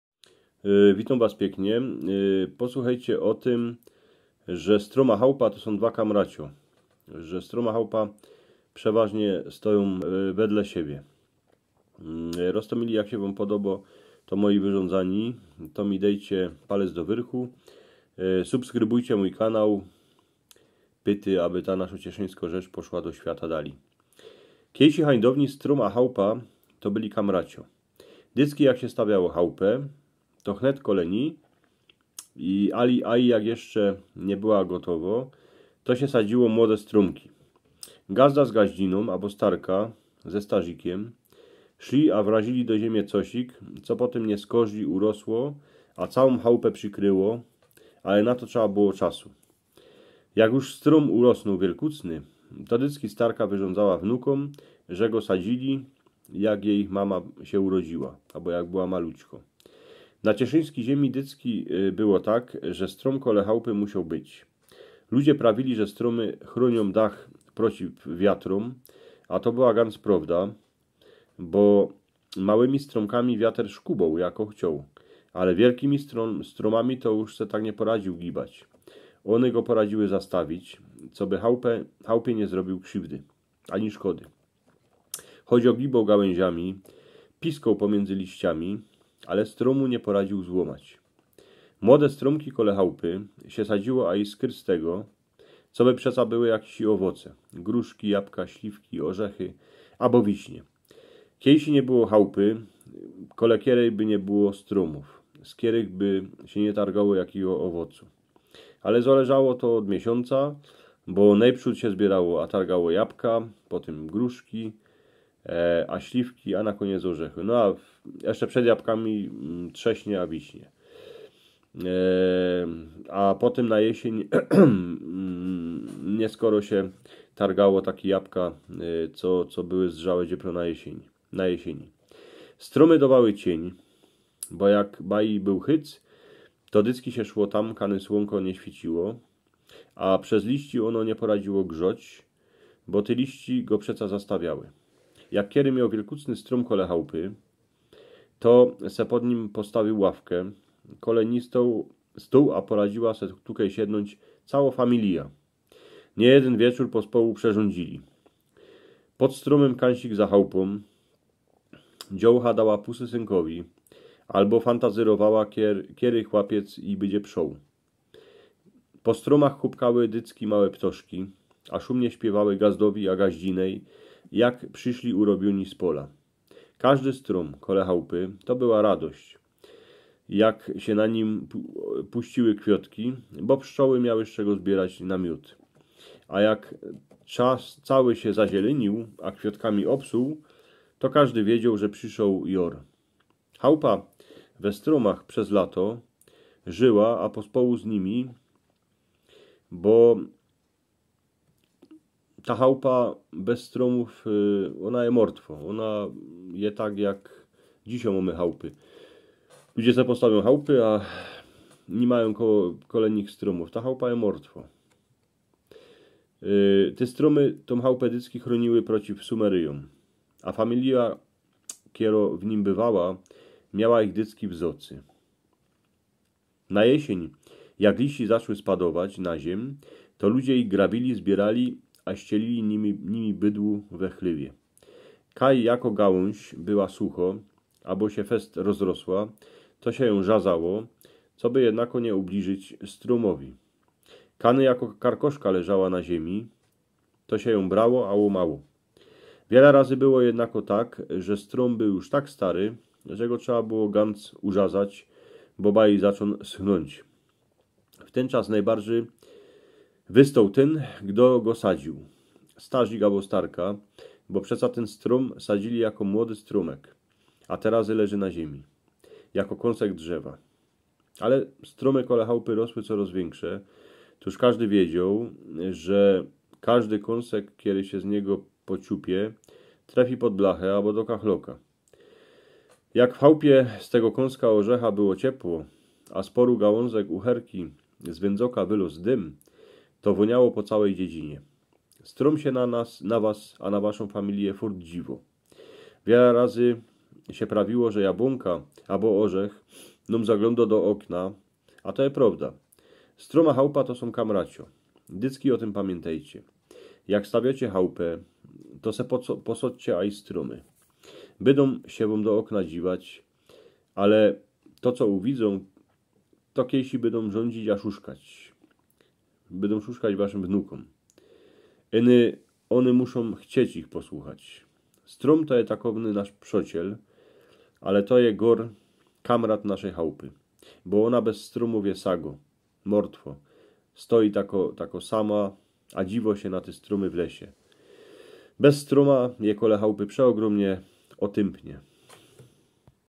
Stróm a chałupa [drzewa i dom] gwara Śląska Cieszyńskiego gwara cieszyńska po naszymu
Posłuchejcie o tym, że stróm a chałupa, sóm jak dwo kamracio. Strómy chrónióm chałupe prociw wiatróm a ludzióm dowajóm rostomajte owoce.

W tym odcinku opowiem Wam o tym, że dom i drzewa koło domu są jak przyjaciele. Drzewa chronią dom przed wichurami a ludziom dają owoce i cień. Posłuchajcie jak brzmi gwara Śląska Cieszyńskiego - gwara cieszyńska po naszymu. Język Ziemi Cieszyńskiej.

Pozyskane w ten sposób środki, przeznaczę na opłacenie strony internetowej i ew. zakup mikrofonu, kamerki czy tła do nagrywania kolejnych odcinków.

Dziękuję i zapraszam na moje pozostałe filmy po naszymu!